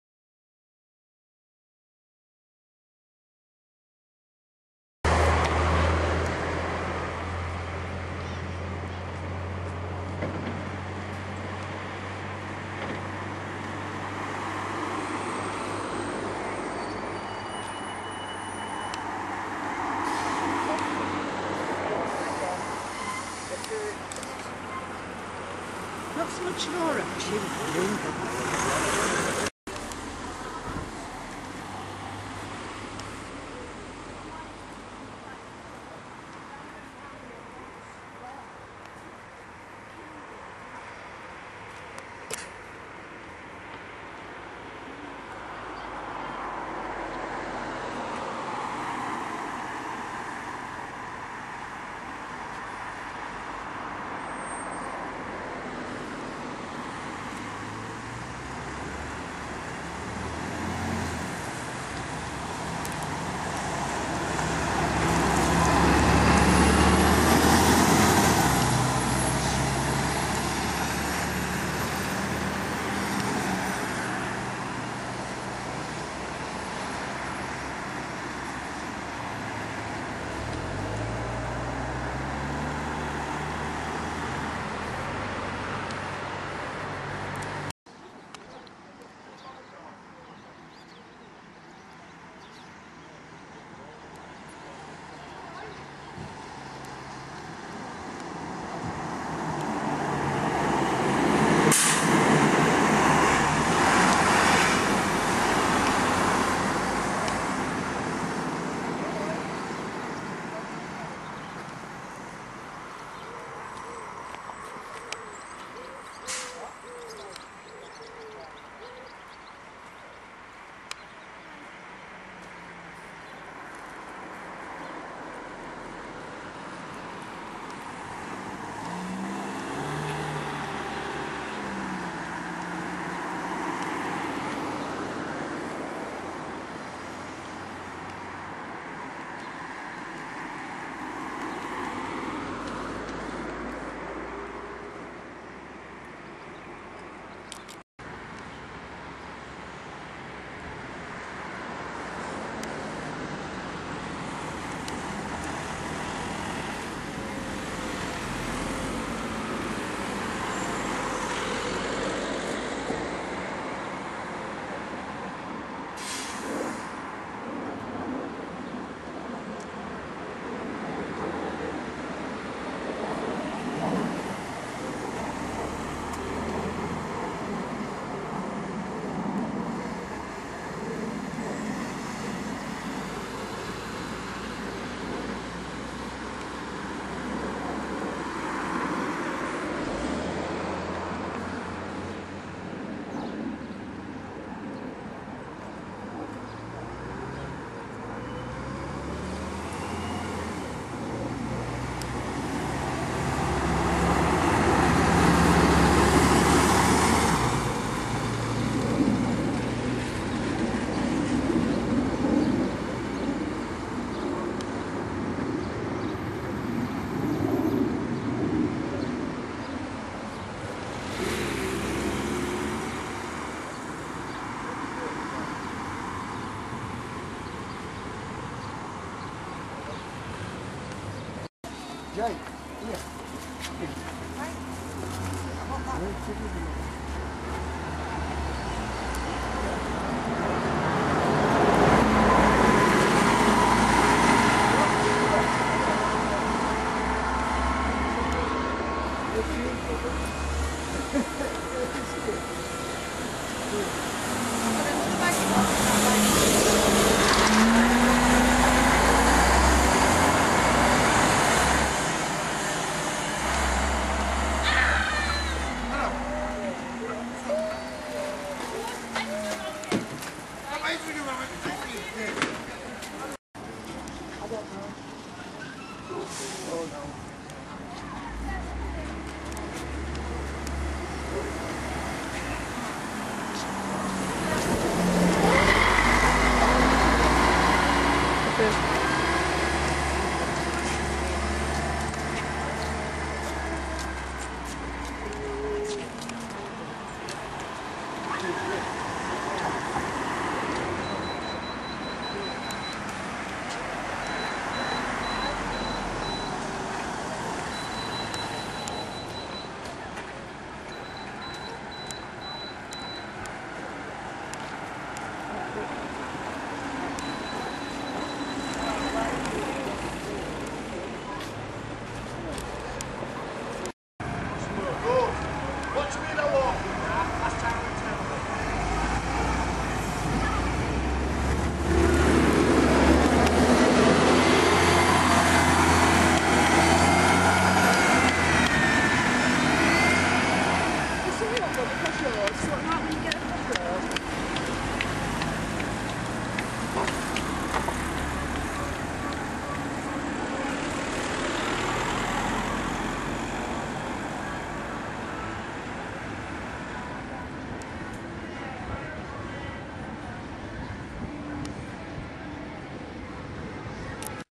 Buses Around Chapel St Leonards And Ingolmells
Here is a few videos of Stagecoach Lincolnshire and HUNTS